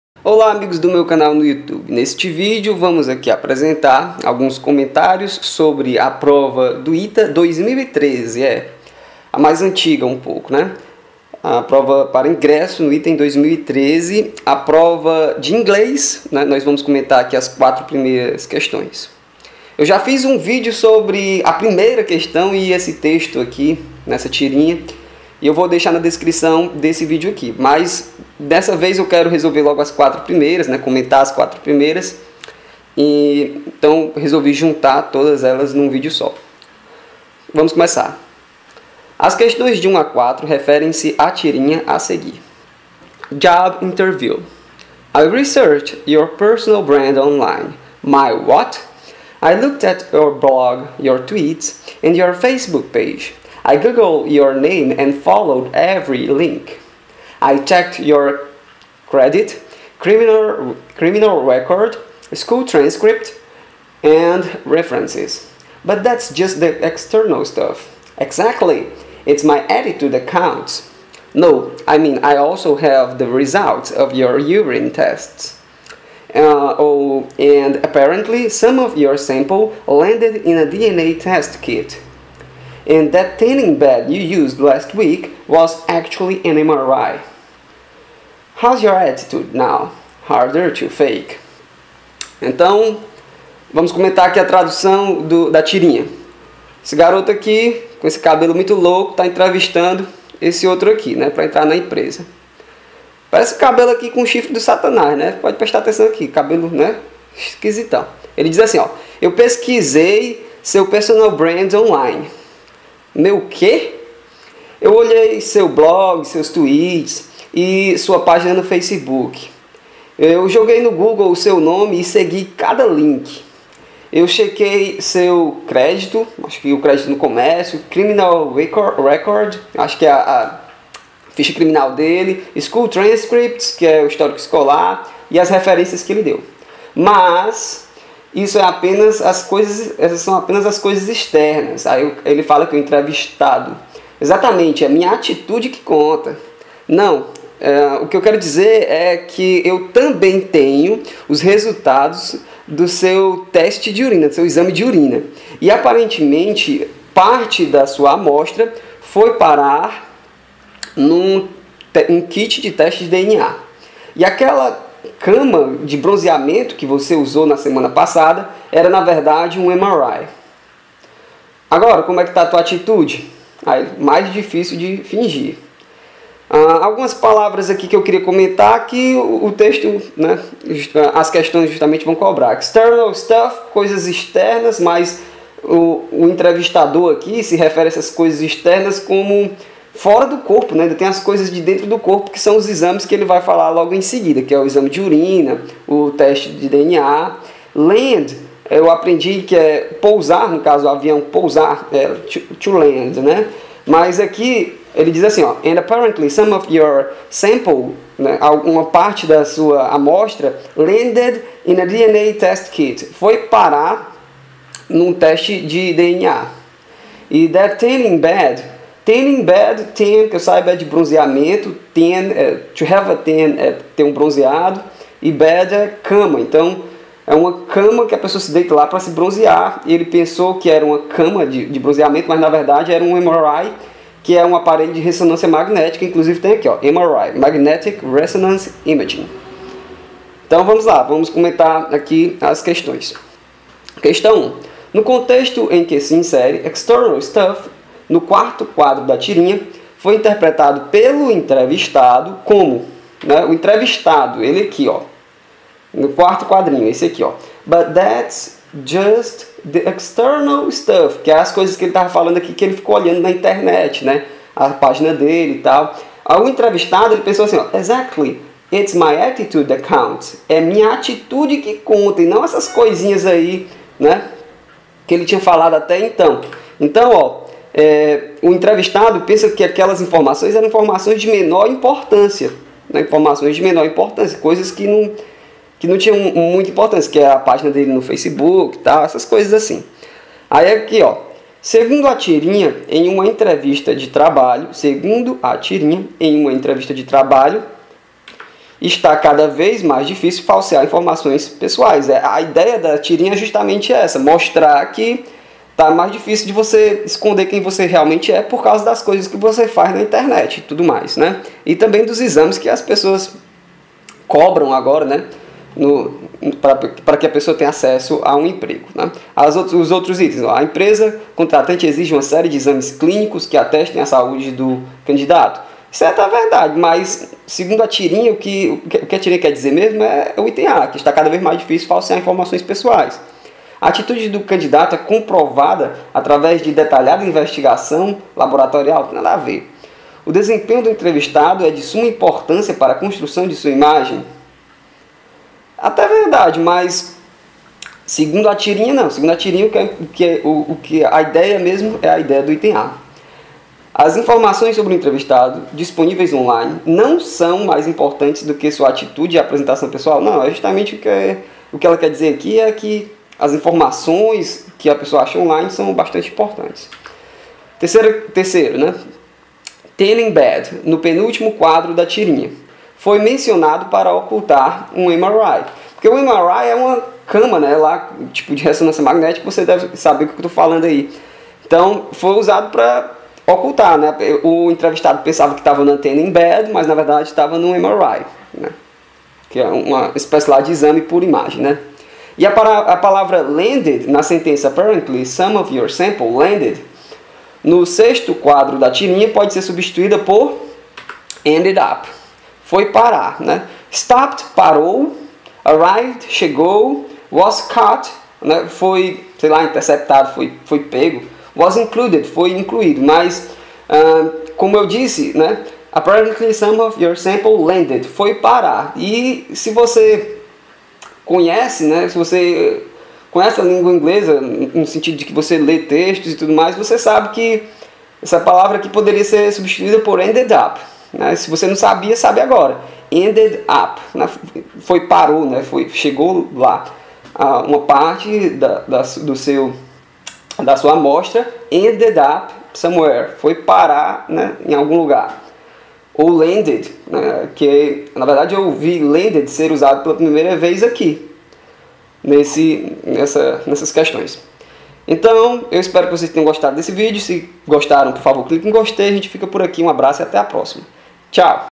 Inglês [ITA-2013, Questões de 1 a 4]
Resolução de algumas questões da prova de Inglês do ITA 2013.